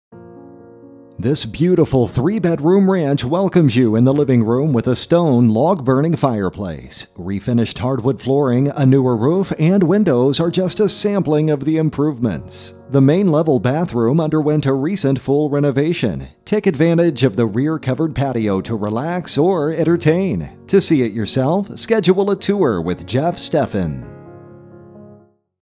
1243 Grouse Drive Pittsburgh, PA | ColdwellBankerHomes.com
1243 Grouse Drive, Pittsburgh, PA

Who says you cant have it all? This beautiful three bedroom ranch is meticulously updated and checks all the boxes for the features that youd love to have in your new home. The living room offers a stone log-burning fireplace. Refinished hardwood flooring, a newer roof, newer windows, newer raised